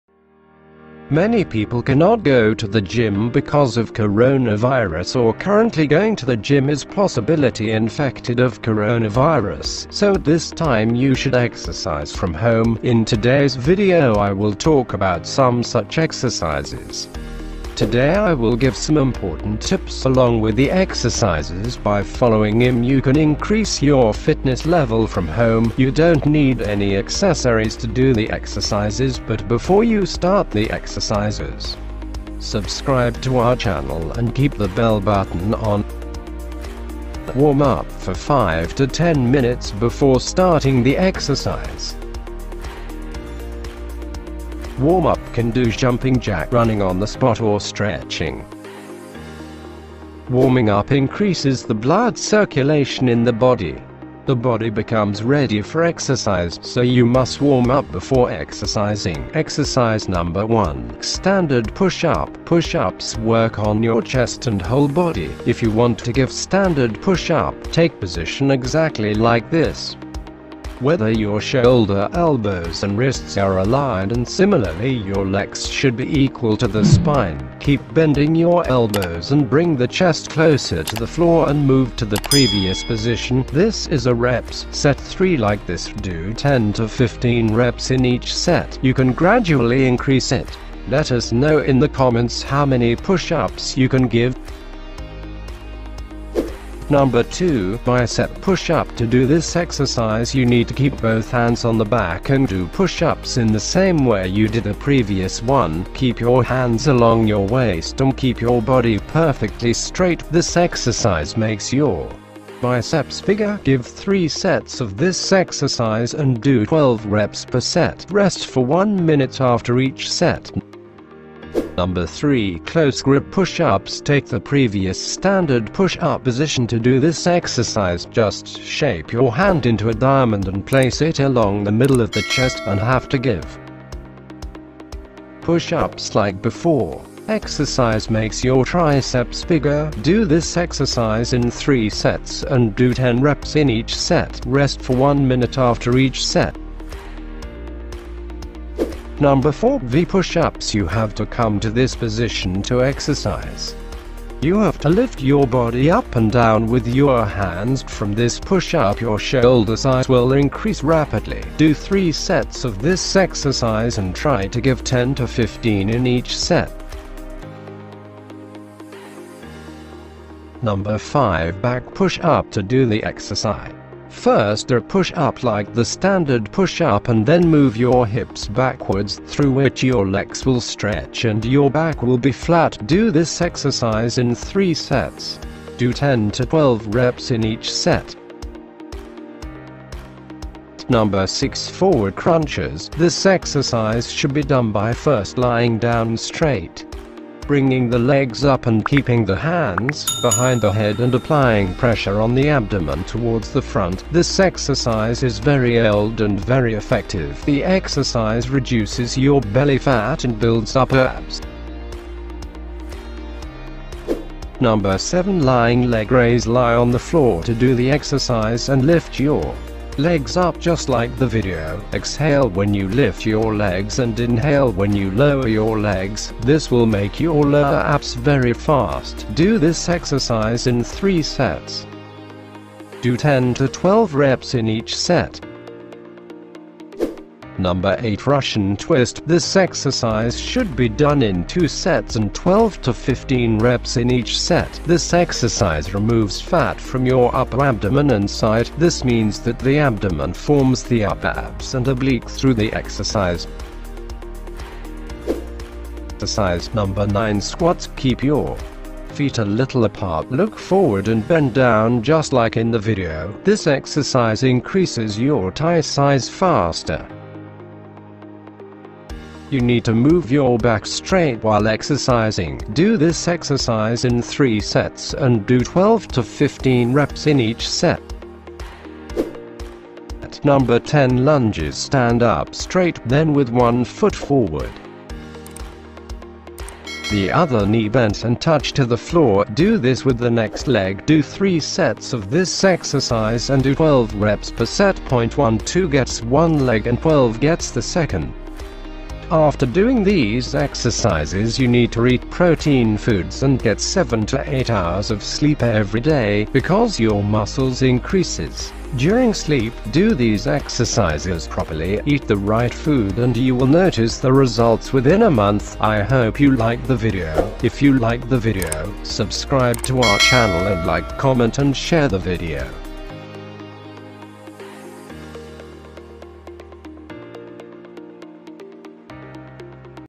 Full Body workout at Home | No Equipment
In today's video I will talk about some such exercises and I will give some important tips along with the exercises. By following them you can increase your fitness level from home. You don't need any accessories to do the exercises.

*** we change our channel name

Timestamps⏰ -

0:00  introduction

0:37  Warm up

1:01 Exercise number 1: Standard pushup.

1:36 Number 2: Bicep push up

2:02 Number 3: close grip push ups

2:29 Number 4: v push ups

2:50 Number 5: back push up

3:11  Number 6 - forward crunches

3:34 Number 7 - lying leg raise

3:57 Number 8 - Russian twist

4:15 Number 9 squats

4:38 Number 10 - lunge

5:00 food and sleep

Keywords -
home workout
full body workout routine
No Equipment
full body workout at home without equipment